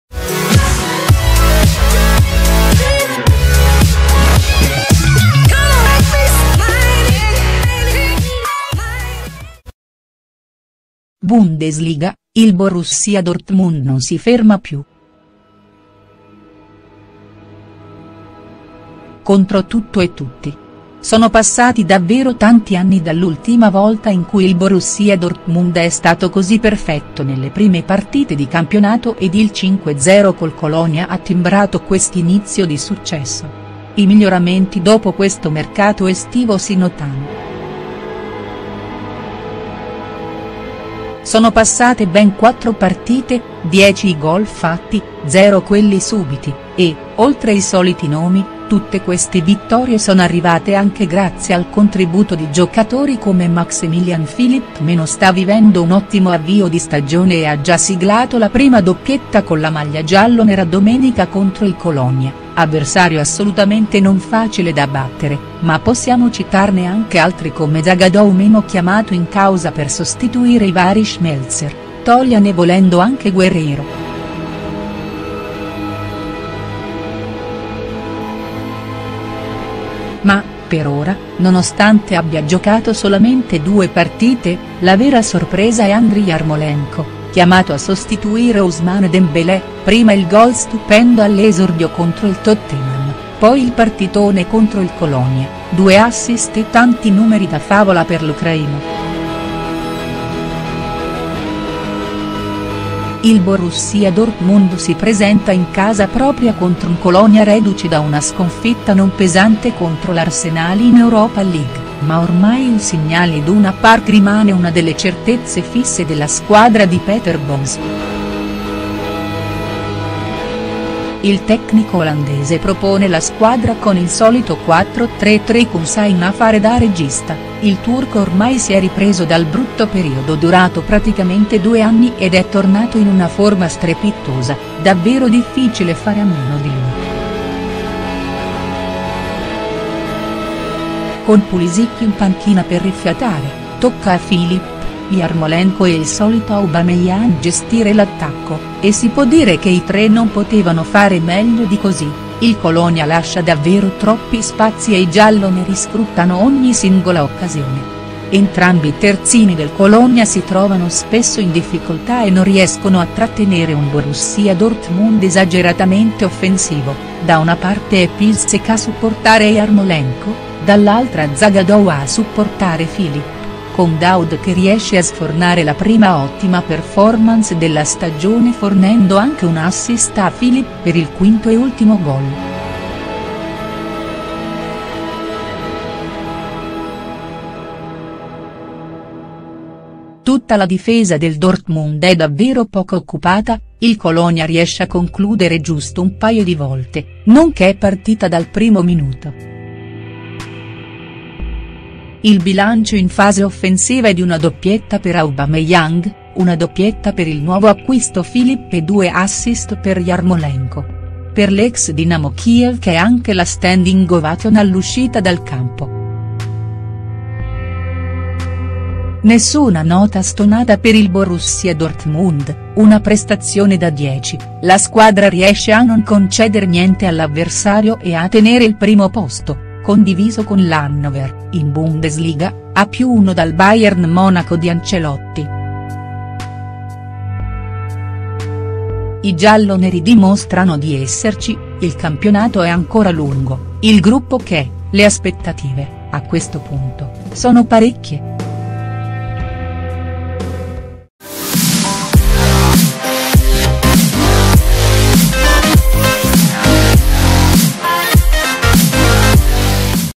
Bundesliga - Il Borussia Dortmund non si ferma più.HD
I gialloneri partono malissimo in Champions League ma continuano a sfornare ottime prestazioni in Bundesliga: fino a che punto possono arrivare?